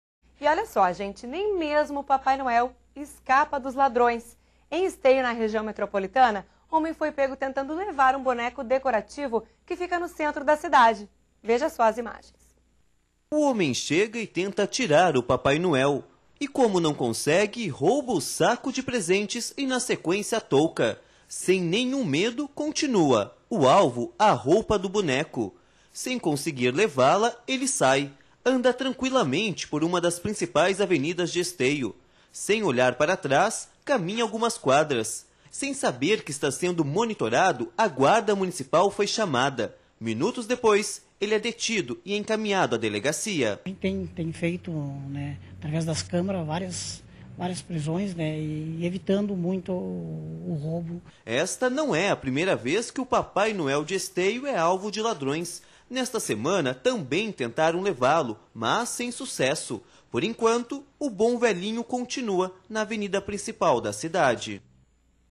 Nem o papai noel escapa
Em
Esteio, na região metropolitana, um homem foi pego tentando levar um
boneco decorativo que fica no centro da cidade.